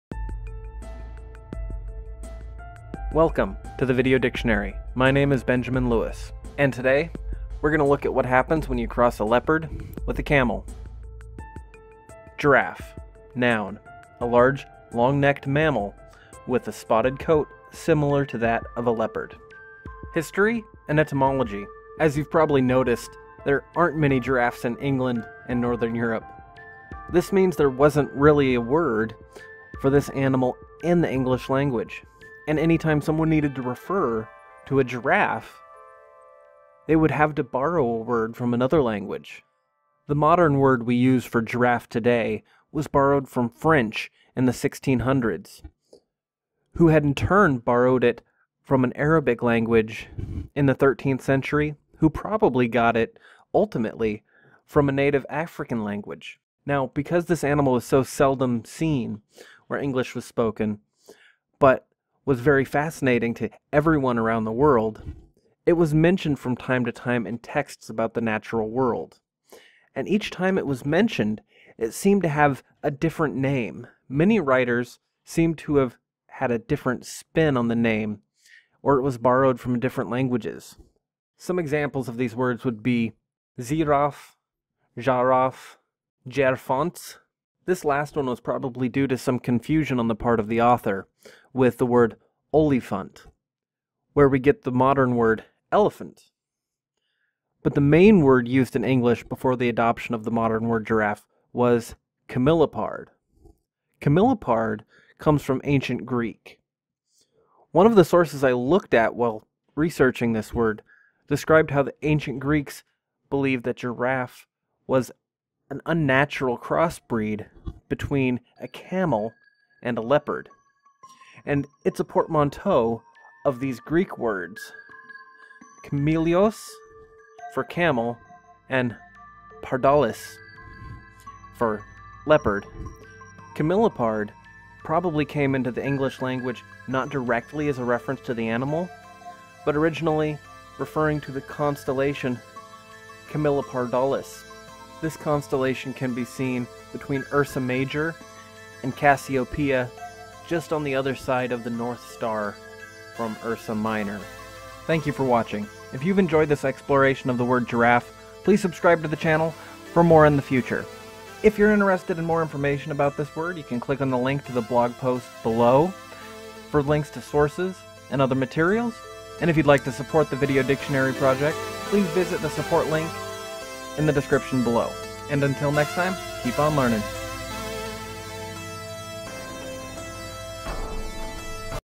Giraffe (n.) - What do you call a camel crossed with a leopard? - The Video Dictionary
What do you call a camel crossed with a leopard? Apparently it’s a...
Giraffe

noun
A large long necked mammal with a spotted coat similar to a leopard.